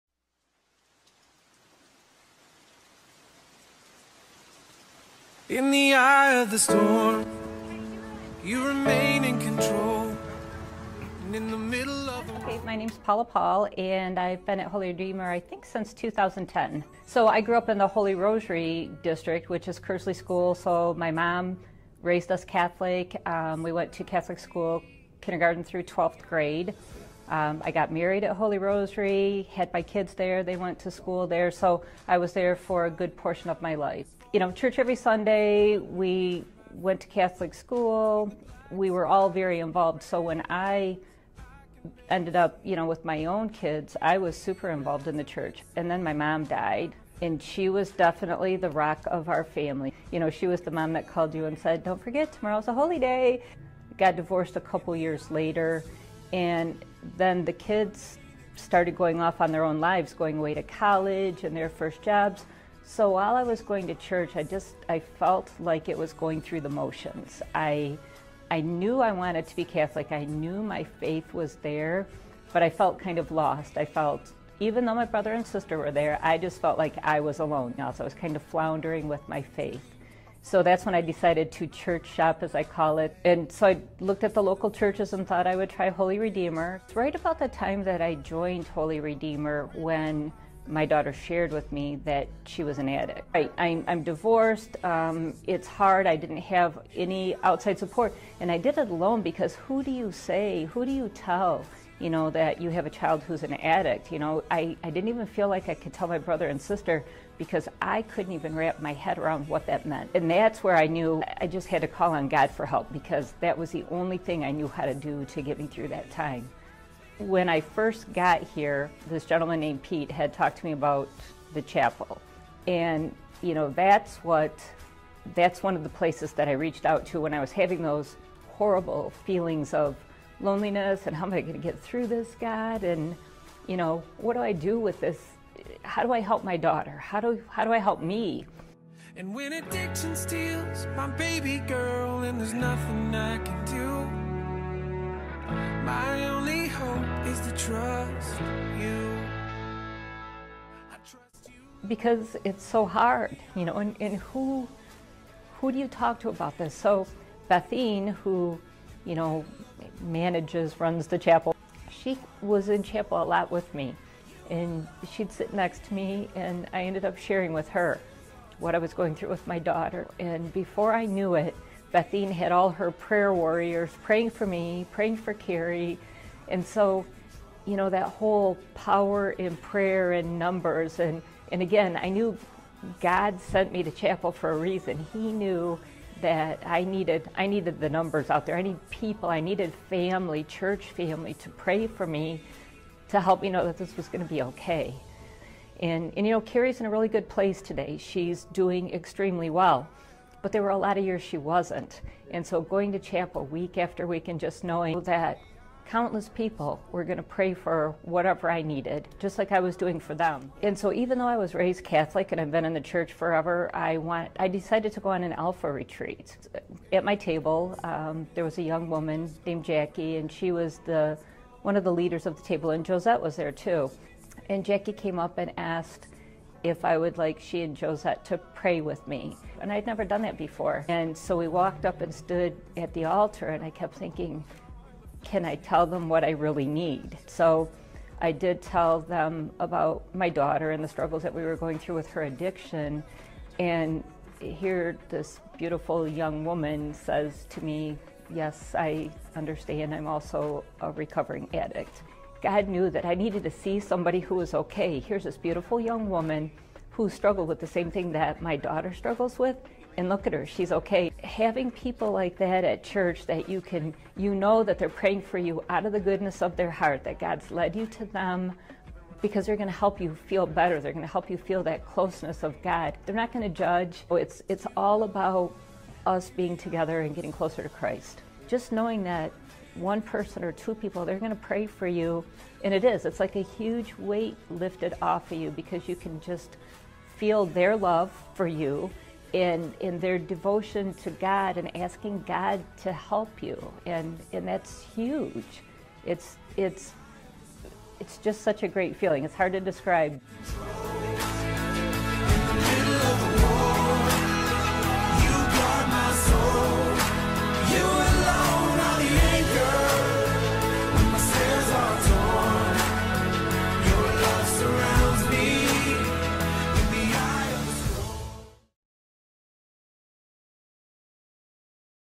Paula Paul's Story | HRC Testimonies | October 31
In this series, we are sharing testimonies from our HRC family! Watch Paula Paul's beautiful story of how she experienced a welcoming community and the power of prayer at HRC! 🙏🏼

Like and follow our pages to receive daily encouragement & prayer!